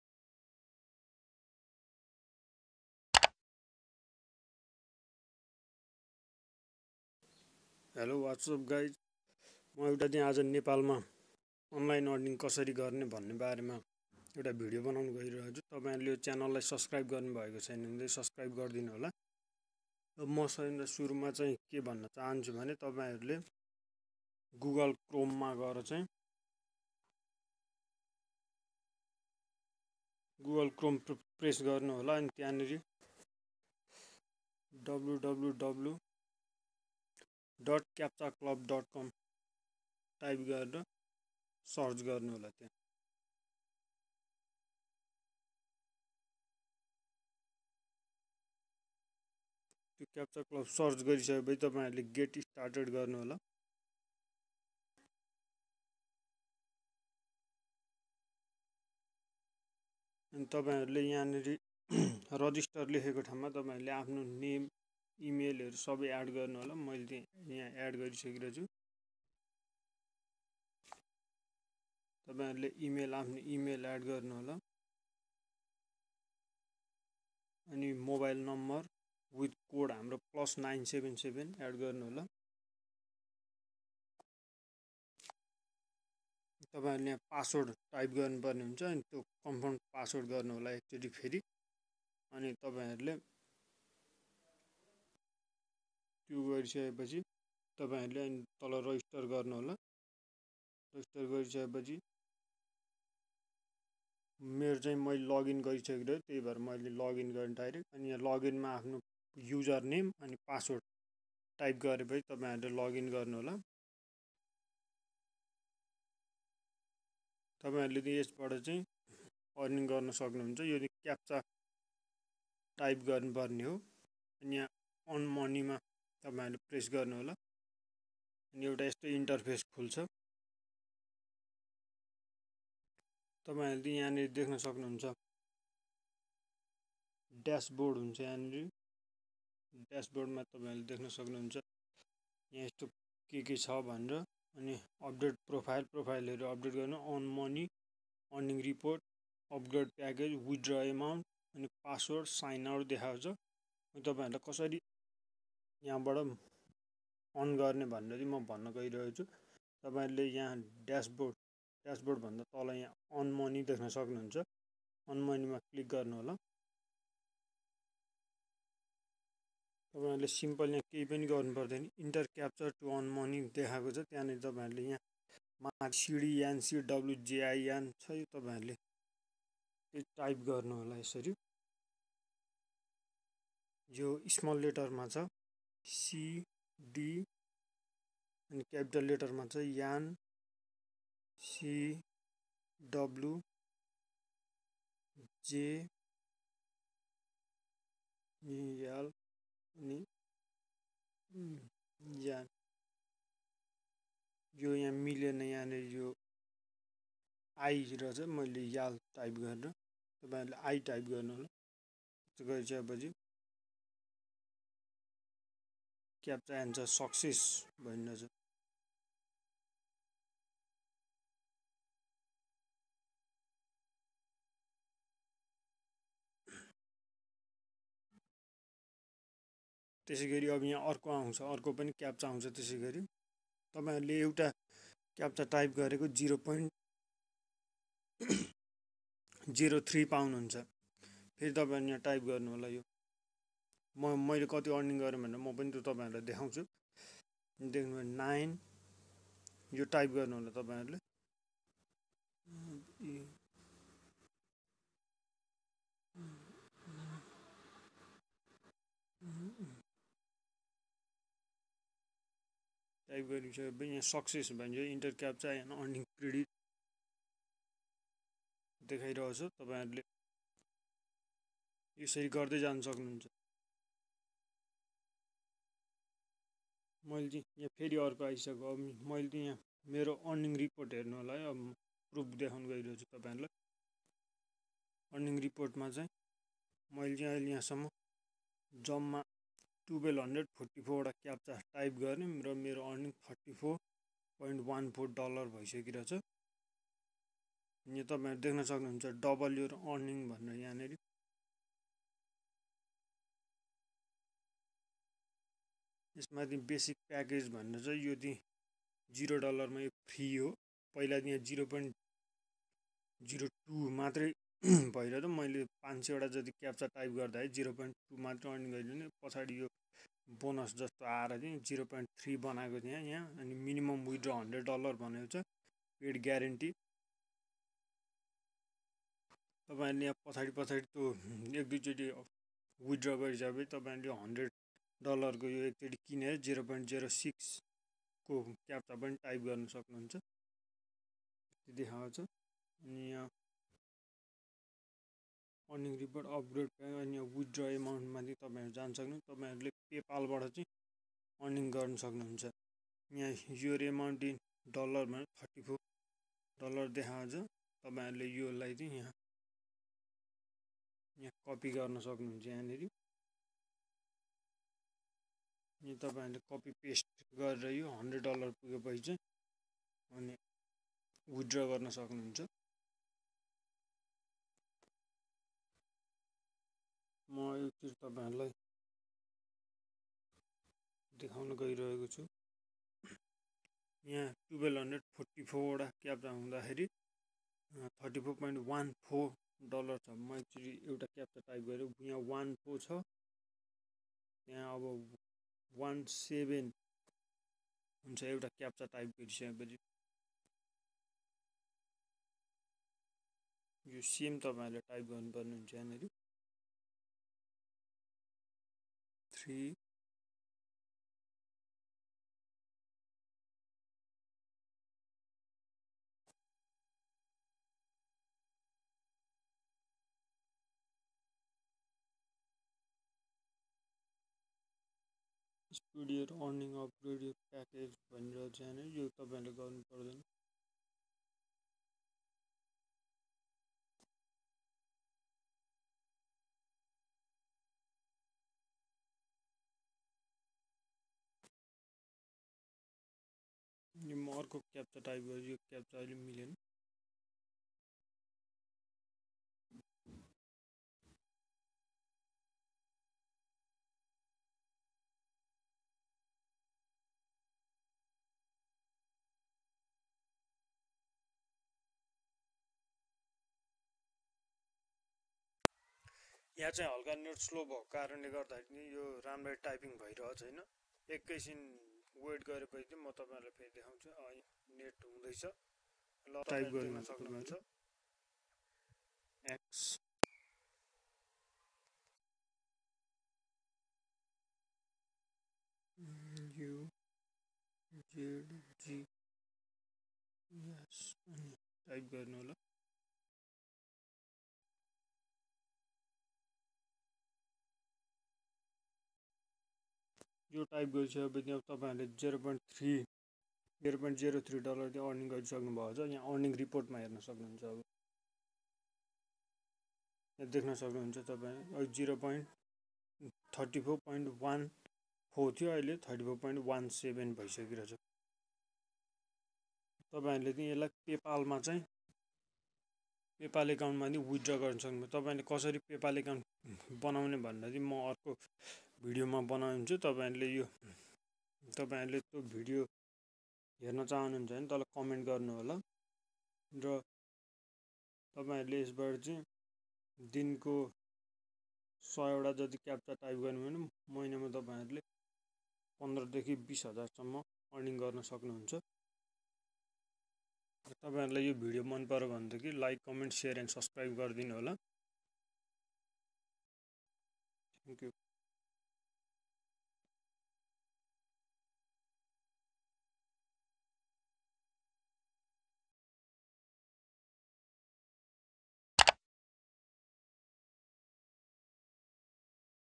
How to earn Rs 15000 online working in 1 Month in nepal
Earn online money in nepal
15000 thousand in one month by captcha typing